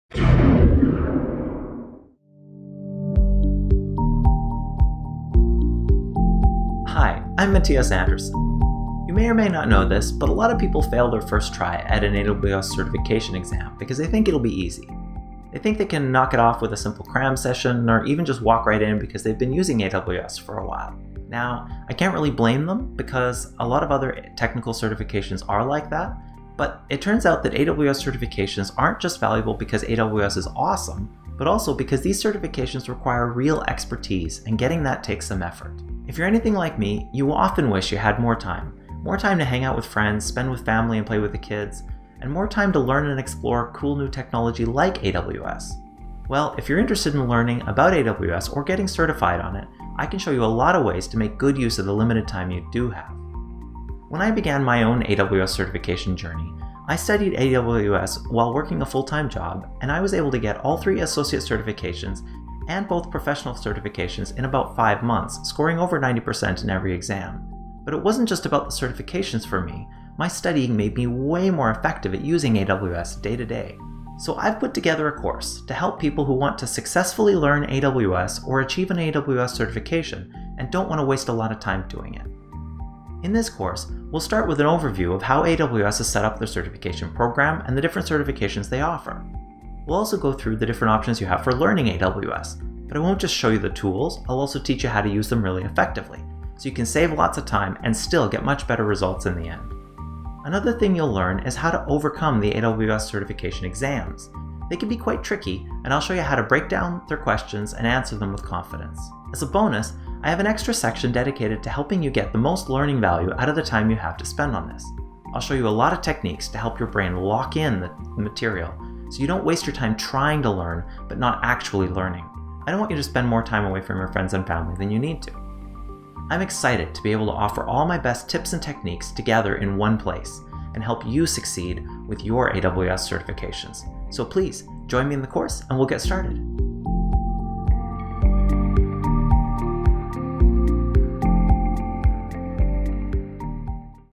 AWS Certification Preparation Guide Promo
Save yourself time, and start studying the right way to learn AWS or pass your AWS certification. This AWS Certification Prep Guide will provide you with the tips and tools you need to succeed.

This course helps you prepare for all AWS certifications -- including associate, professional and specialties. These tips, tricks and strategies apply to all certification exams.